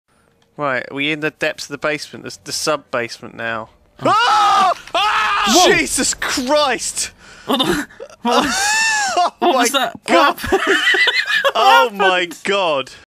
Yogscast Best Screams Ever Episode 2!
I guess Lewis is very scared...

Sorry for the video quality, I did render it in 720p..